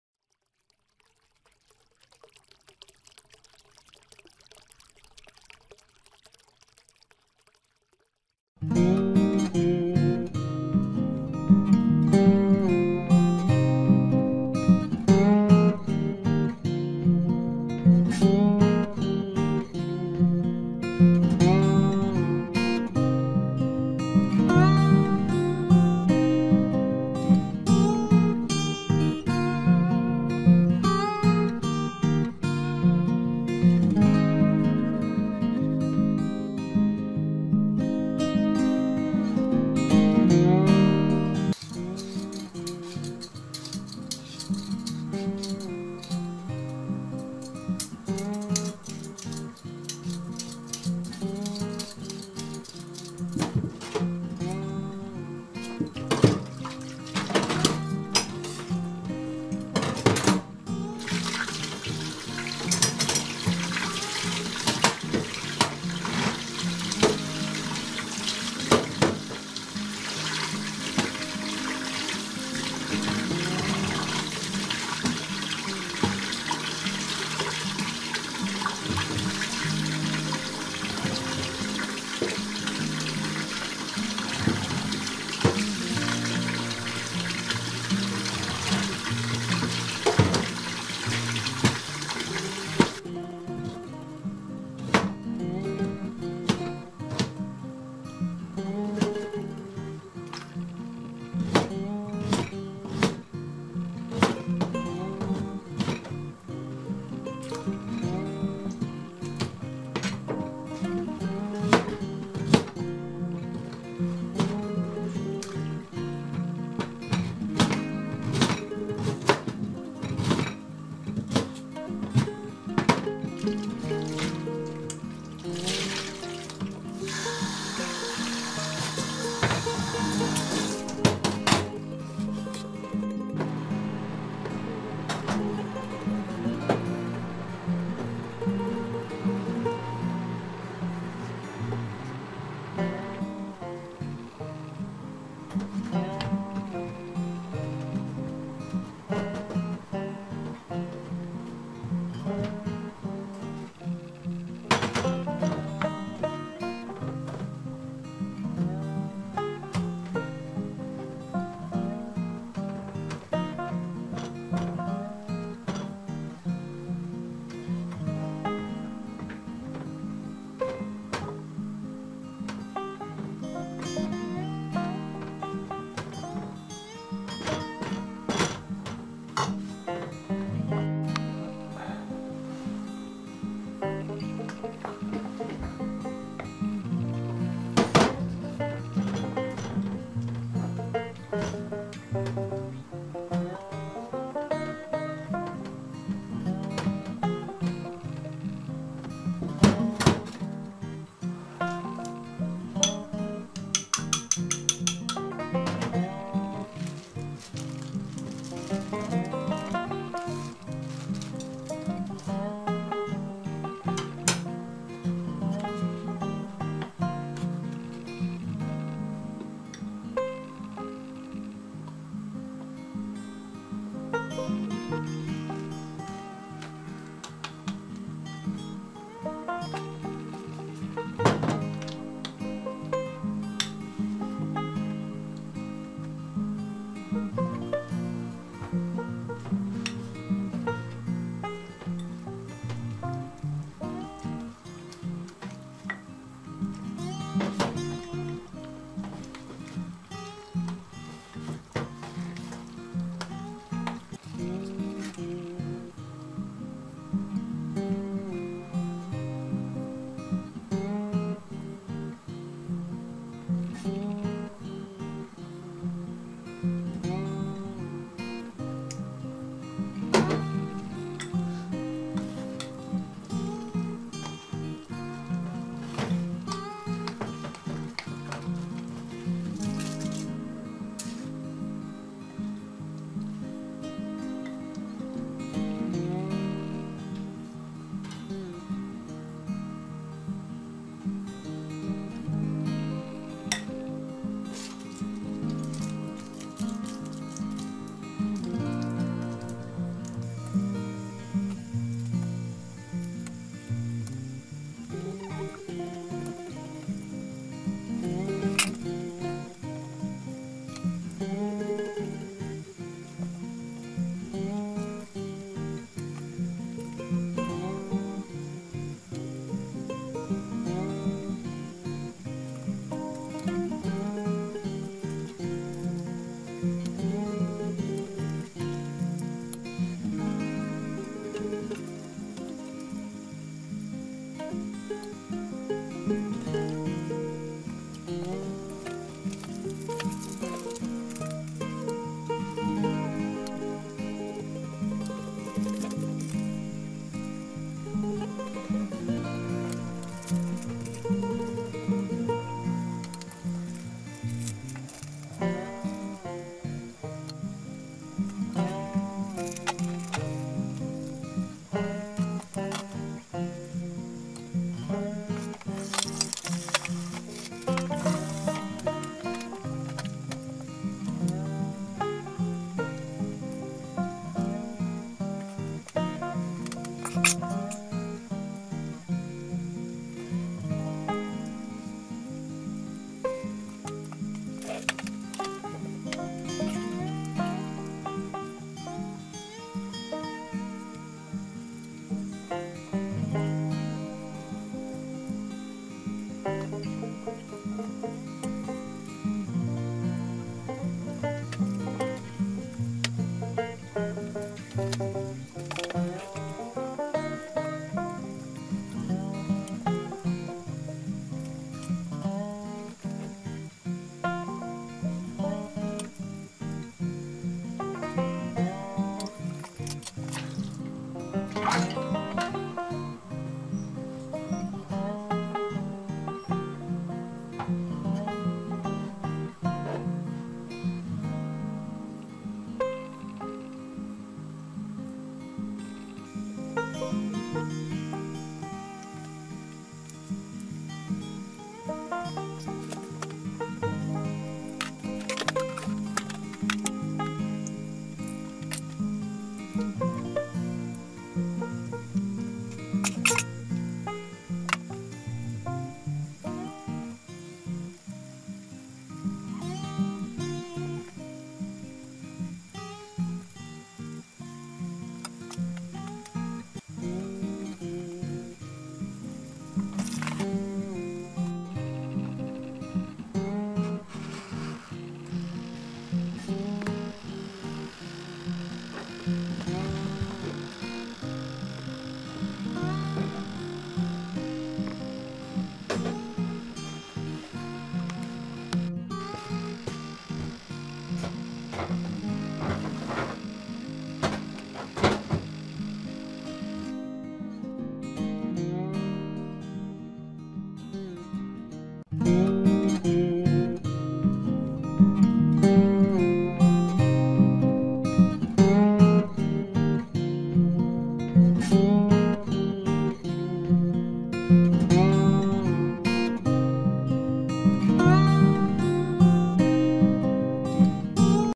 スイ－トポテト作り sweet potato making
材料
サツマイモのぶつ切り600g、バタ－50g、砂糖50g、
生クリーム40g、卵2個、バニラエッセンス5滴程度(好みによる)

サツマイモで作るお菓子。
結構おいしい。

Ingredients: 600g of chopped sweet potato, 50g of butter, 50g of sugar,
40g of whipped cream, 2 eggs, 5 drops of vanilla essence (according to your taste).

Sweets made with sweet potato.
Quite delicious.

設備屋の適当田舎暮らし